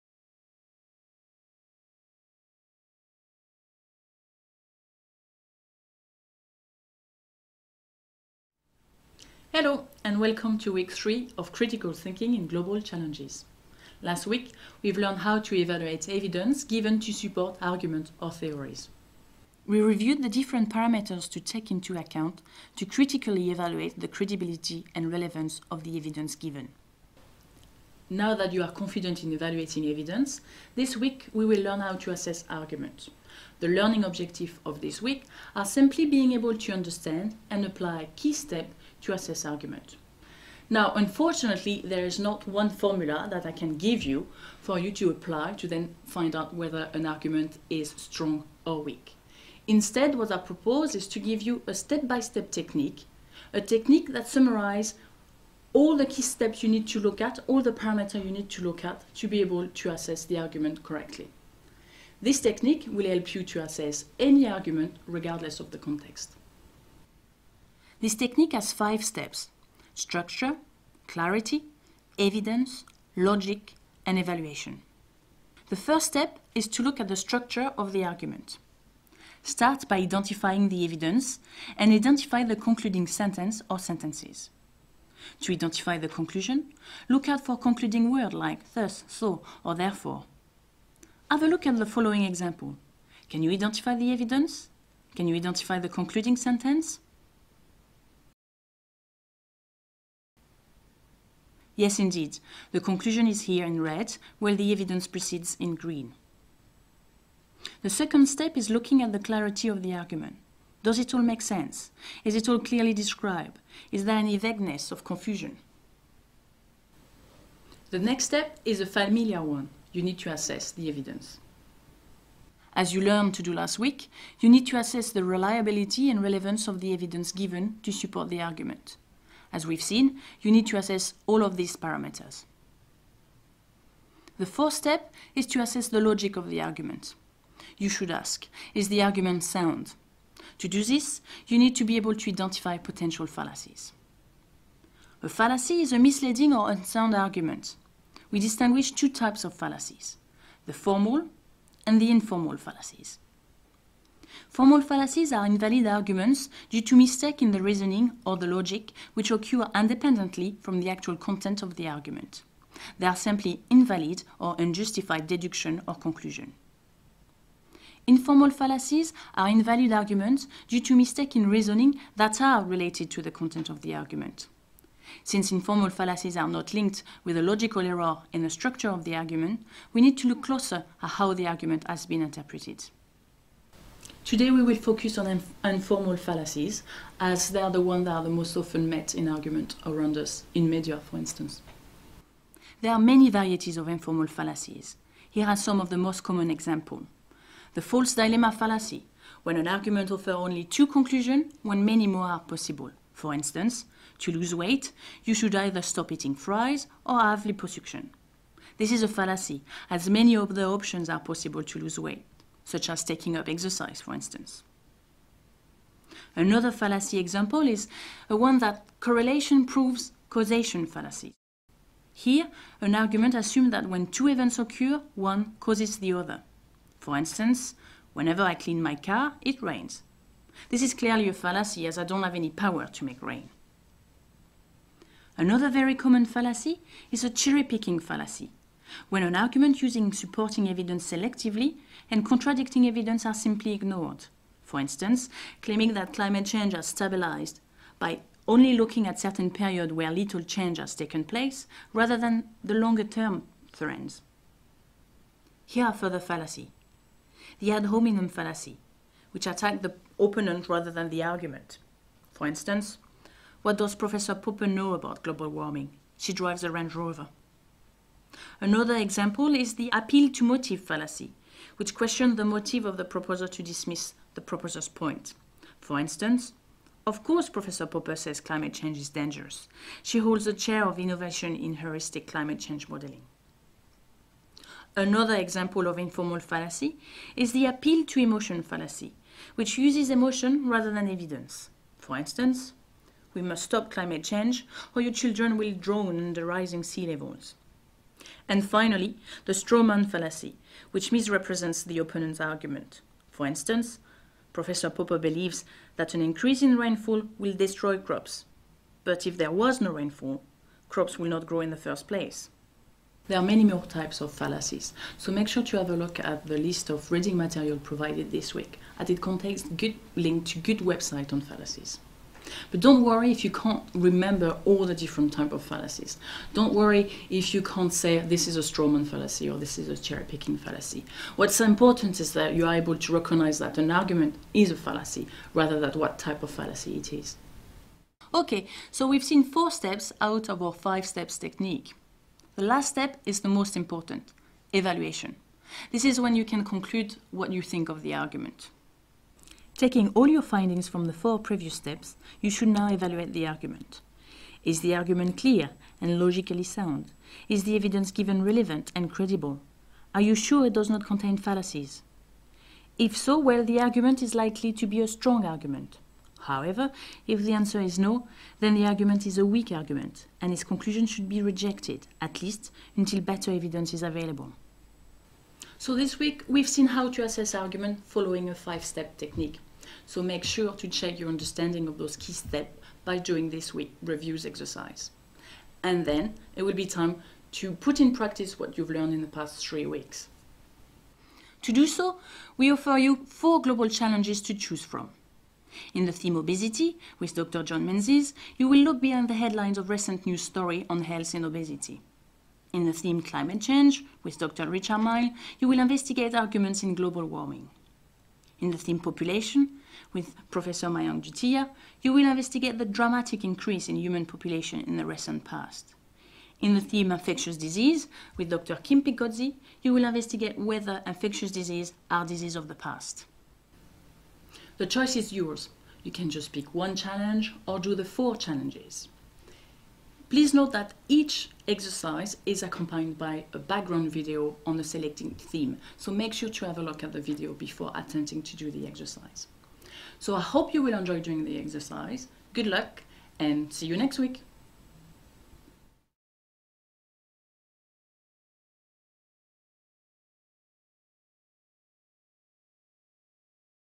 3.1. CT - Assessing Arguments (Part A)
Week three of the University of Edinburgh's "Critical Thinking in Global Challenges" (CT) open online course.

Prof. Mayank Dutia and Dr Celine Caquineau
School of Biomedical Science